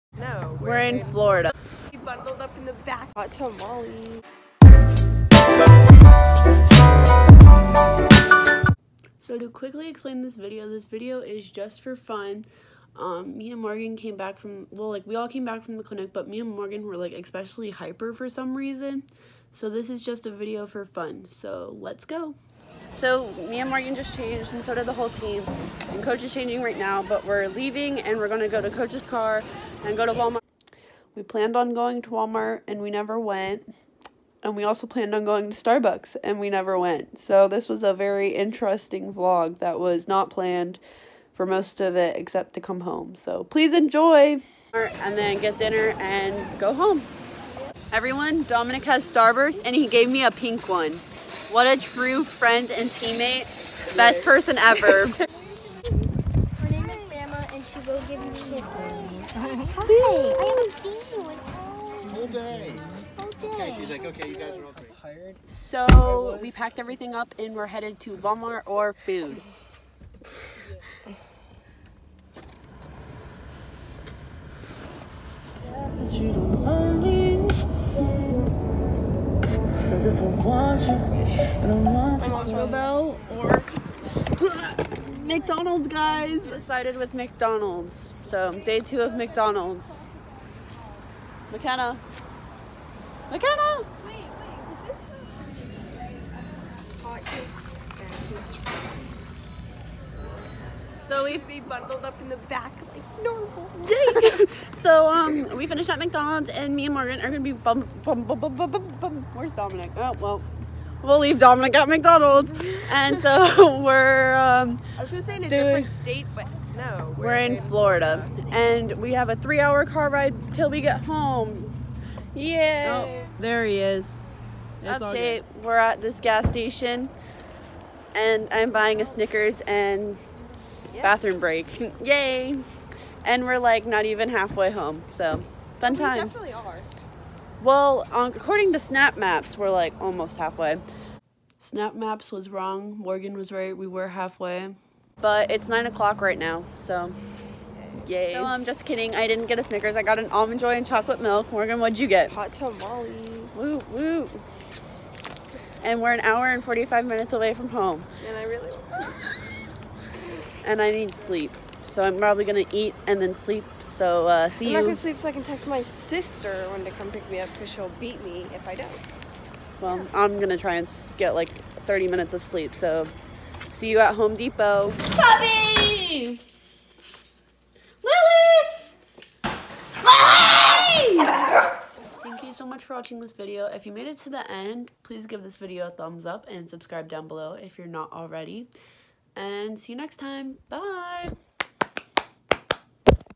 road trip with my besties!!
Last Video-

Hope you like this video of Morgan and I on our way back from the FSU clinic (go watch it, link up above)!!